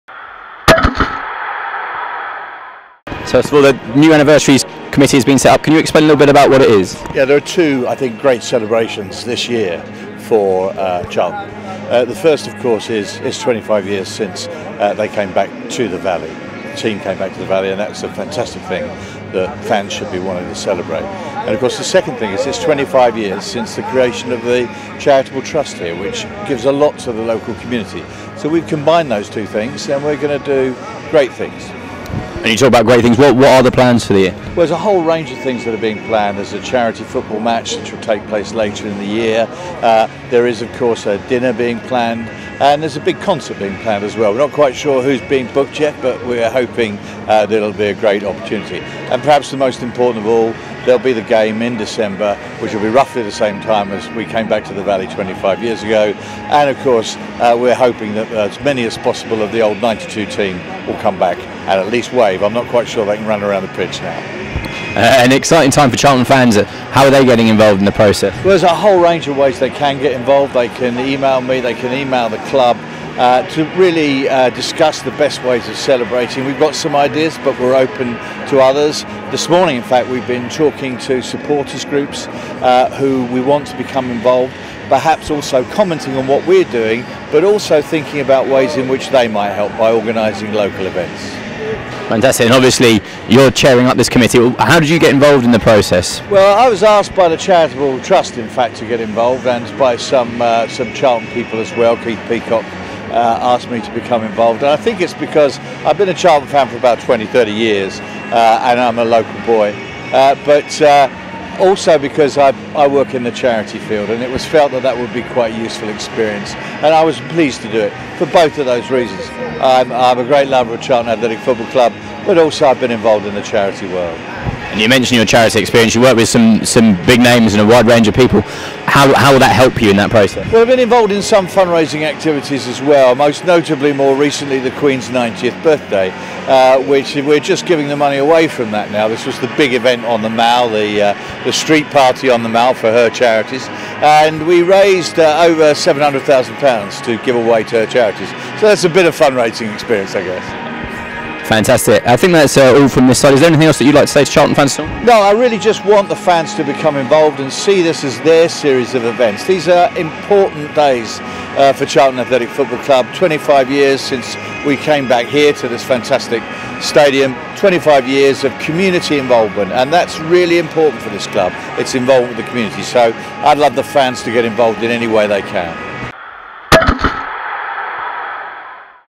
Sir Stuart Etherington looks ahead to 2017's 25-year celebrations
Sir Stuart Etherington, Chair of the 25th Anniversary Committee, is encouraging fans to give their input on ideas to celebrate the anniversaries of both the return to The Valley and the creation of the Charlton Athletic Community Trust (CACT).